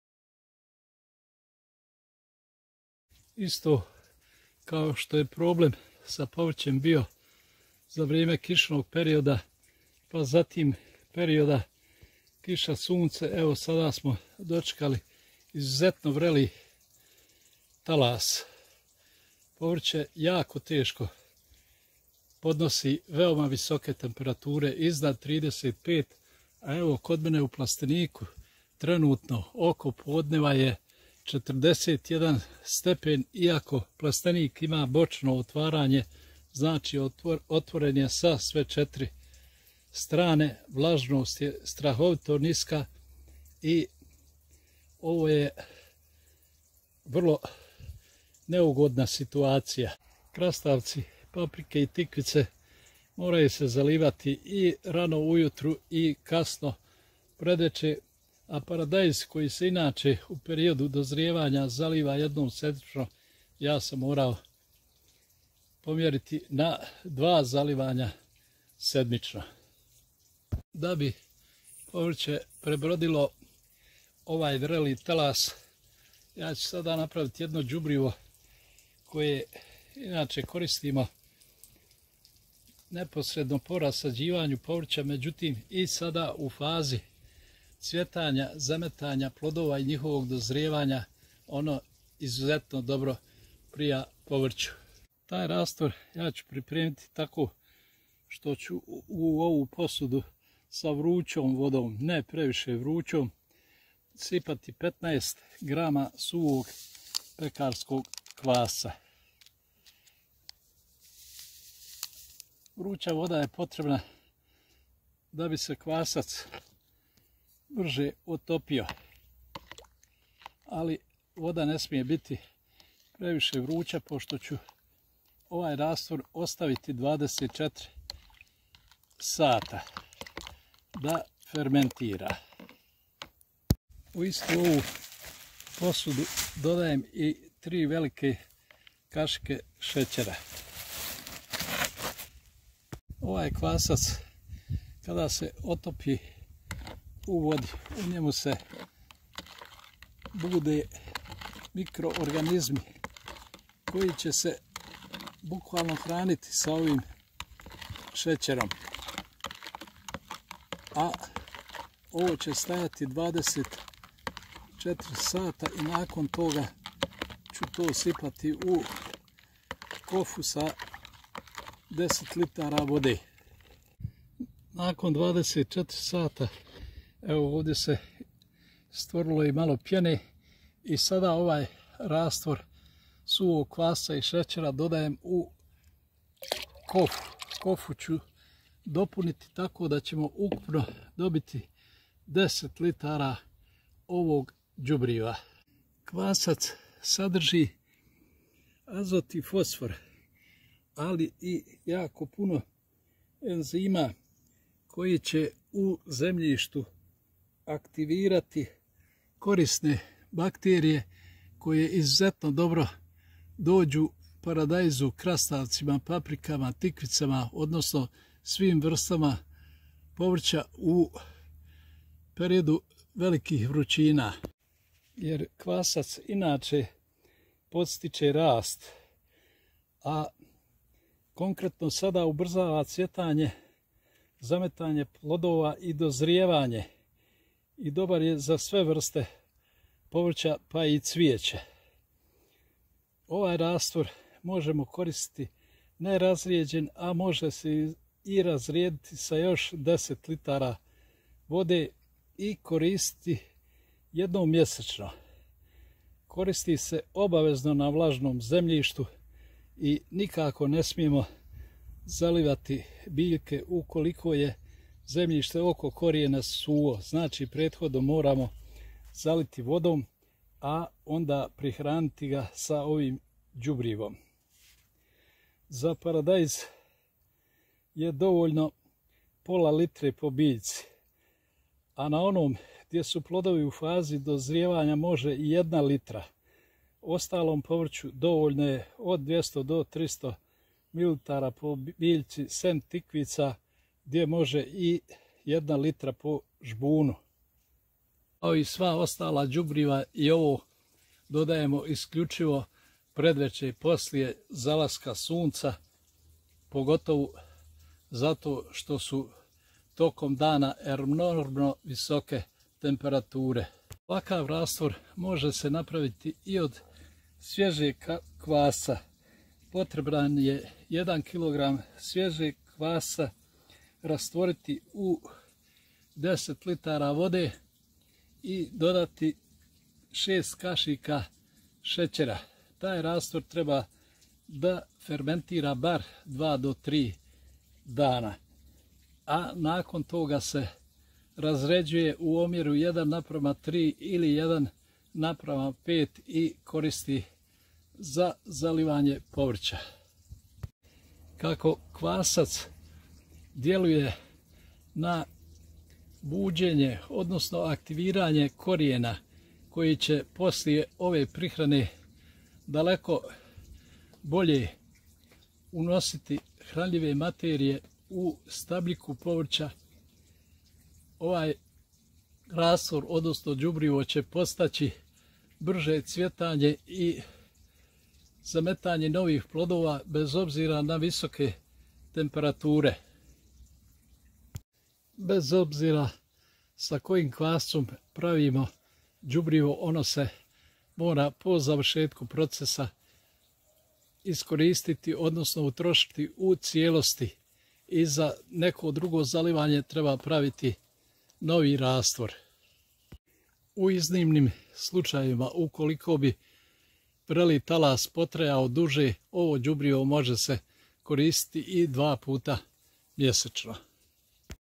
Hitno dodajte svom povrću ovo đubrivo da ne osjeti vreli talas
@  Temperature iznad 35 C su jako nepovoljne za sve vrste povrća i neke vrste gotovo potpuno staju sa aktivnostima i treba ih hitno prihraniti ovim đubrivom,